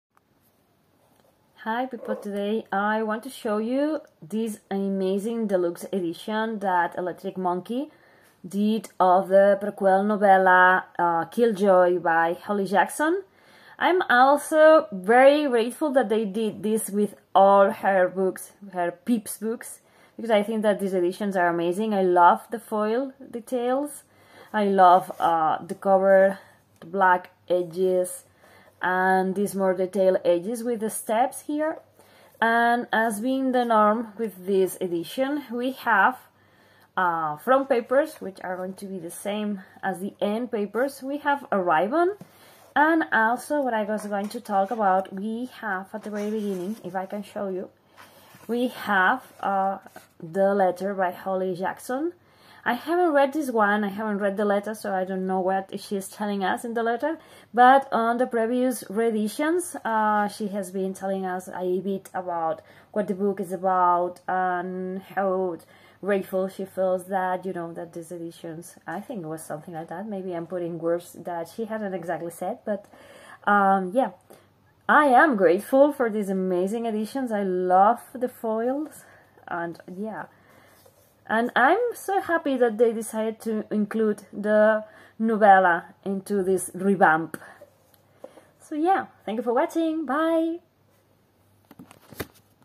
Kill Joy by Holly Jackson
Find out where it all started for Pip in this prequel novella to the A Good Girl's Guide to Murder.

Pippa Fitz-Amobi is not in the mood for her friend's murder mystery party. Especially one that involves 1920's fancy dress and pretending that their town, Little Kilton, is an island called Joy. But when the game begins, Pip finds herself drawn into the make-believe world of intrigue, deception and murder. But as Pip plays detective, teasing out the identity of the killer clue-by-clue, the murder of the fictional Reginald Remy isn't the only case on her mind ... Contains mature content, for older readers.